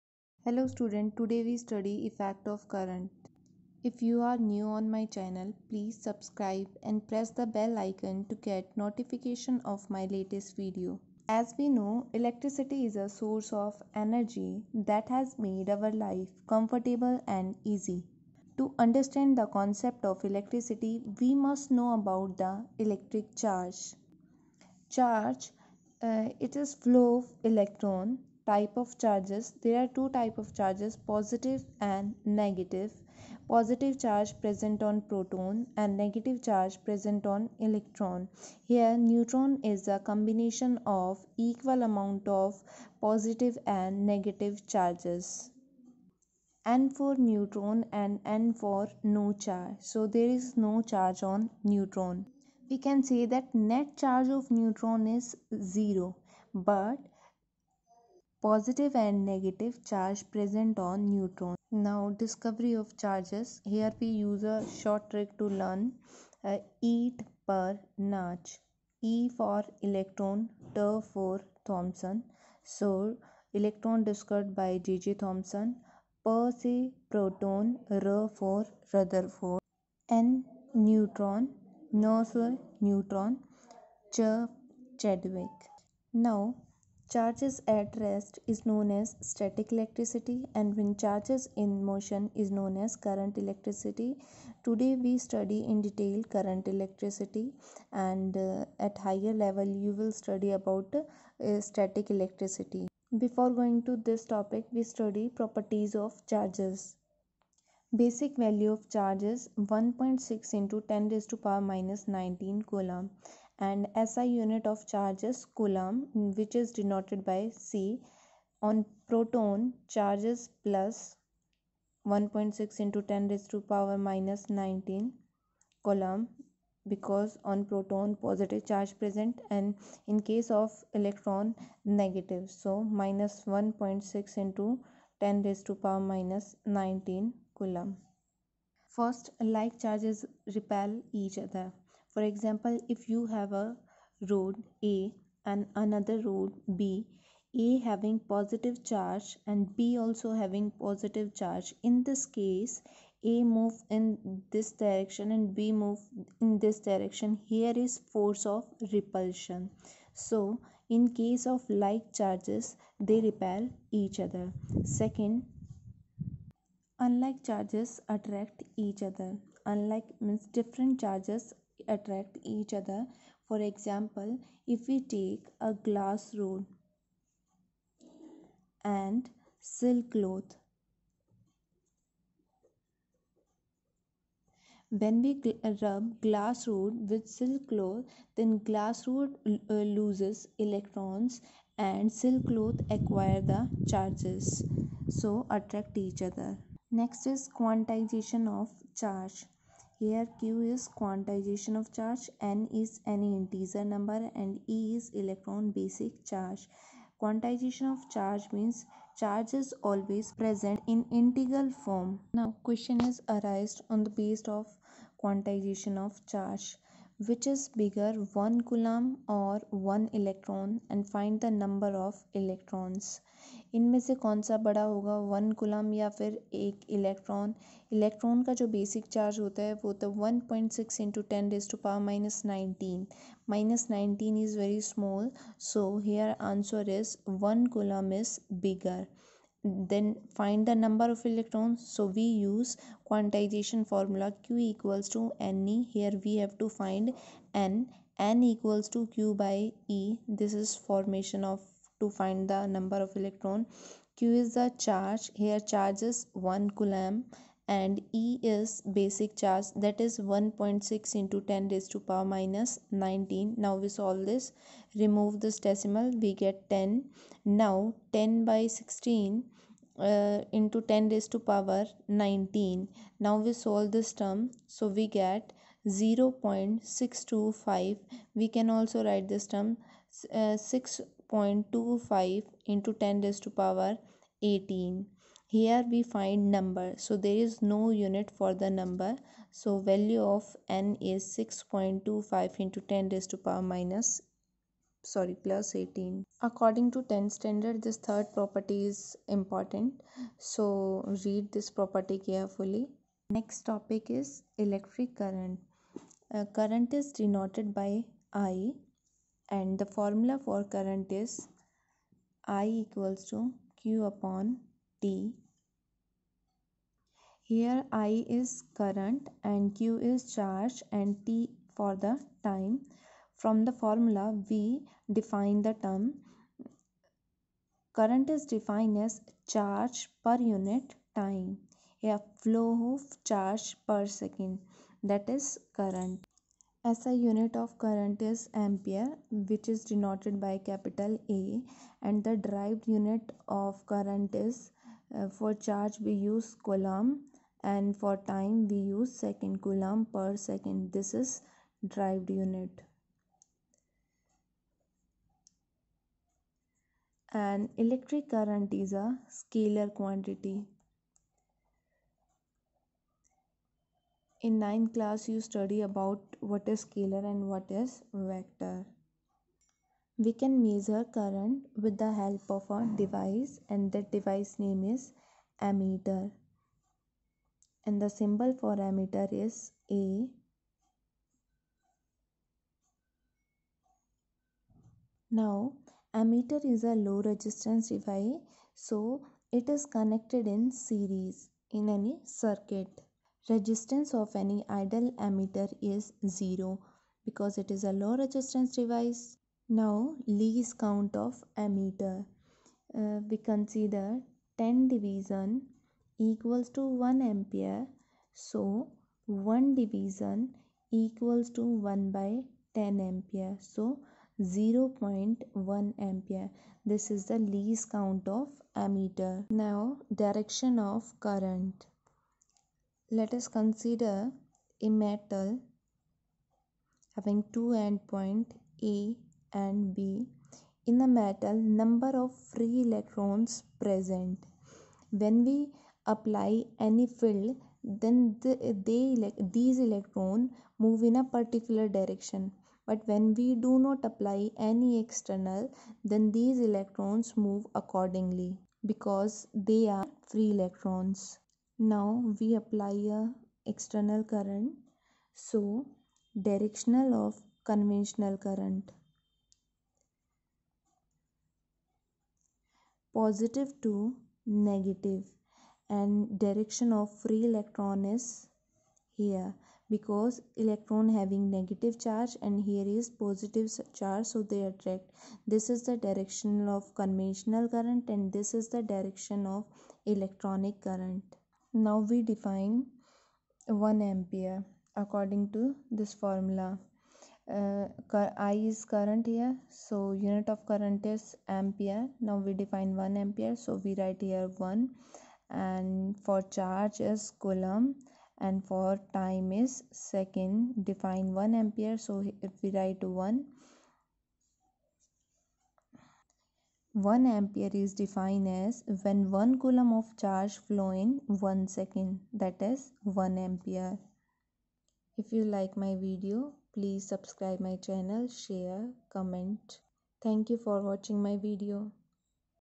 Effect of current (class-10th) Charge & Current detailed explanation -01
CLASS- XTH

CH-1 EFFECT OF CURRENT
TOPIC-  1) CHARGE & IT'S PROPERTIES
               2) CURRENT